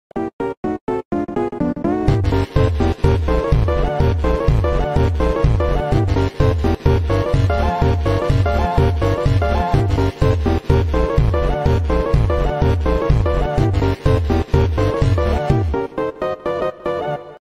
dummy meme||bfb/tpot after the final au
this is a wholesome au that I will mostly use for ships and positive memes

characters

evil leafy (after the final au)

saw (after the final au)

gaty (after the finalau)